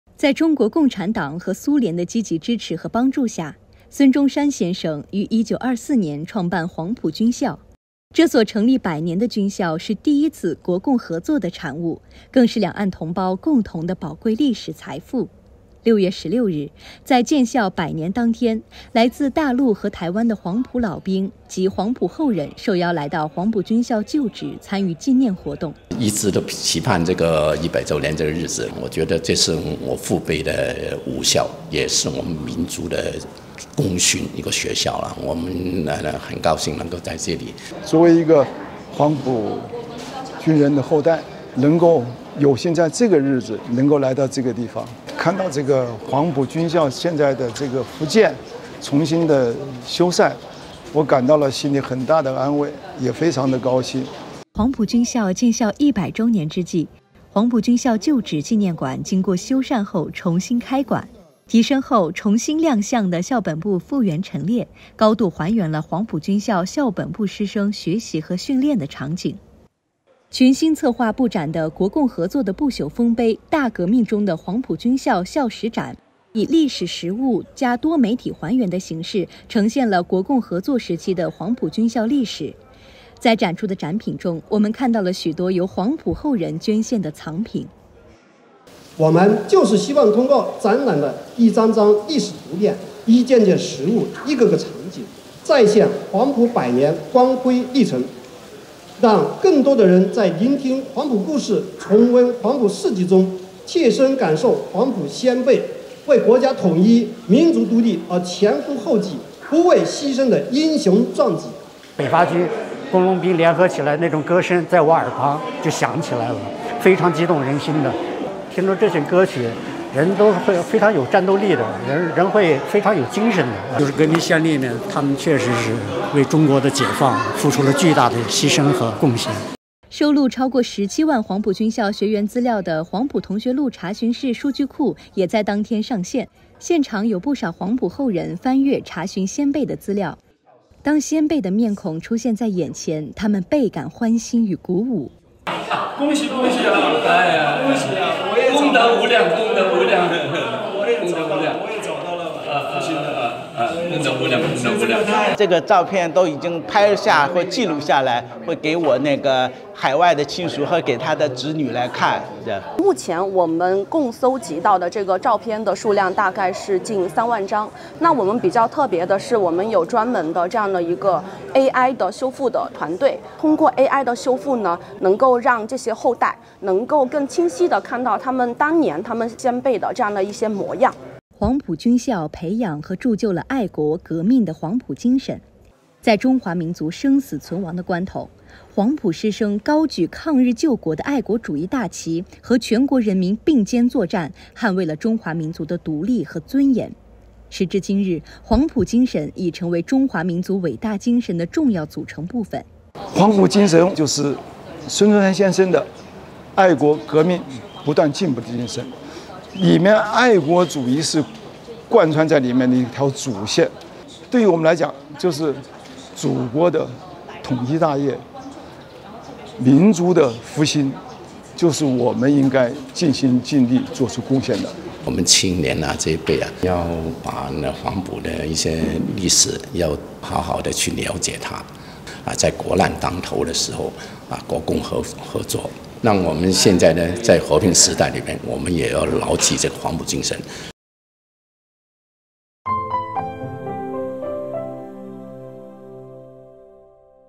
黄埔老兵、后人共同纪念百年黄埔军校 冀传承黄埔精神
在中国共产党和苏联的积极支持和帮助下，孙中山先生于1924年创办黄埔军校。这所成立百年的军校是第一次国共合作的产物，更是两岸同胞共同的宝贵历史财富。6月16日，在建校百年当天，来自大陆和台湾的黄埔老兵及黄埔后人受邀来到黄埔军校旧址参与纪念活动。

在黄埔军校建校100周年之际，黄埔军校旧址纪念馆经过修缮后重新开馆。提升后重新亮相的校本部复原陈列，高度还原了黄埔军校校本部师生学习和训练的场景。全新策划布展的“国共合作的不朽丰碑——大革命中的黄埔军校（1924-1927）”校史展，以“历史实物+多媒体还原”的形式，呈现了国共合作时期的黄埔军校历史。在展出的展品中，我们看到了许多由黄埔后人捐献的藏品。

收录超过17万黄埔军校学员资料的“黄埔同学录查询室”数据库也在当天上线。现场有不少黄埔后人翻阅查询先辈的资料，当先辈的面孔出现在眼前，他们倍感欢欣与鼓舞。

黄埔军校培养和铸就了爱国、革命的黄埔精神。在中华民族生死存亡的关头，黄埔师生高举抗日救国的爱国主义大旗，和全国人民并肩作战，捍卫了中华民族的独立和尊严。时至今日，黄埔精神已成为中华民族伟大精神的重要组成部分。